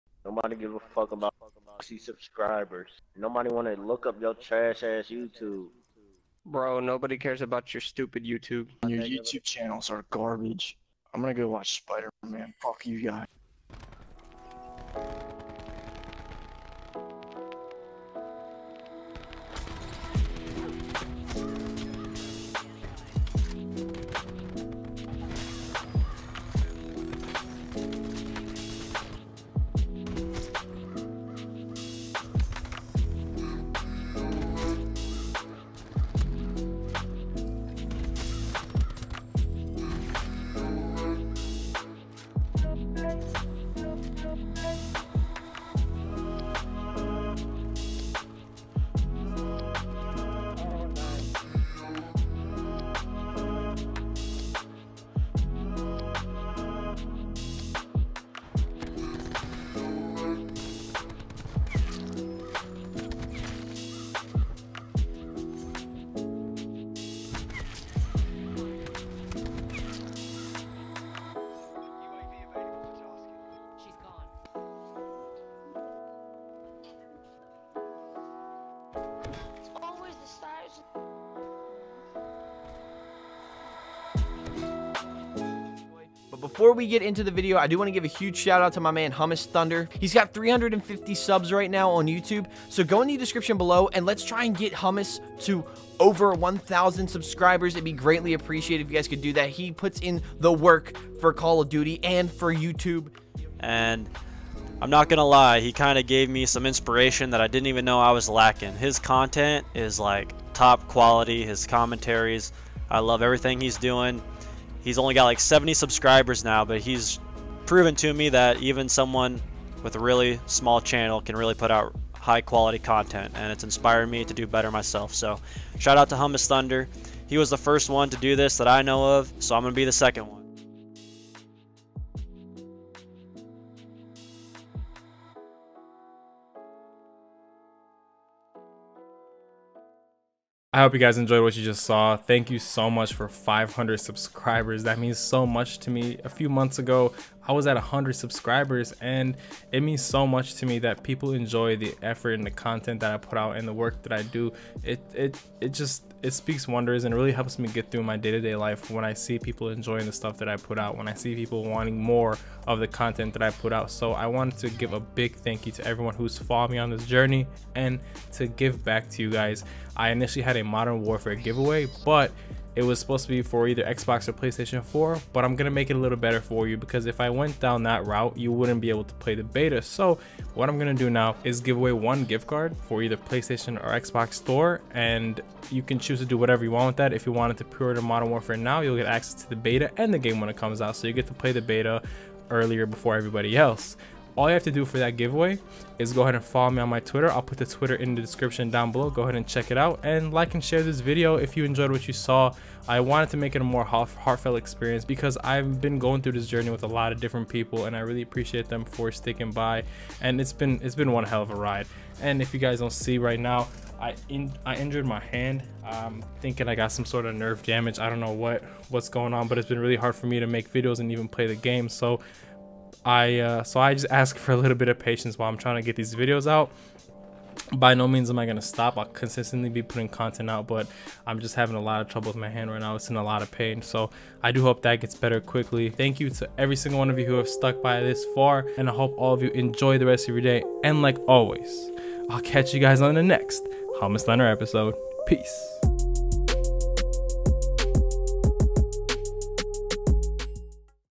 Thank You for 500 Subs! W/ Giveaway!
I just want to say thank you from the bottom of my heart for the consistent support shown on this channel. If it wasn't for all 500+ of you, I wouldn't be here doing what I love. I hope you guys enjoy what you see!

Peace!